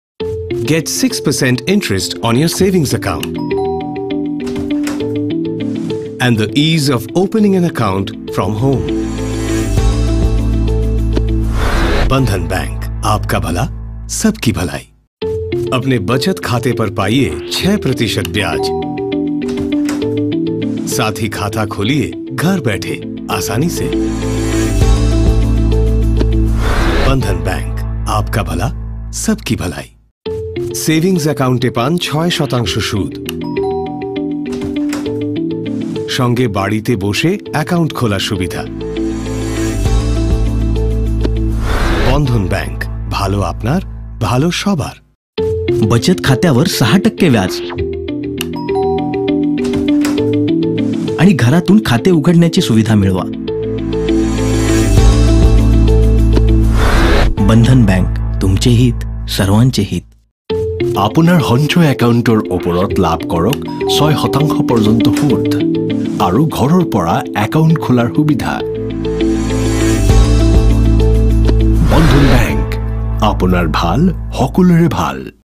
Bandhan Bank TVC | Music, Sound Design & Mix by Sanjib Sarkar | Octave Studio Kolkata
Bandhan Bank TVC | Music, Sound Design & Mix by Sanjib Sarkar |
Language - English, Hindi, Bengali, Marathi & Assamese
Studio - Octave Studio Kolkata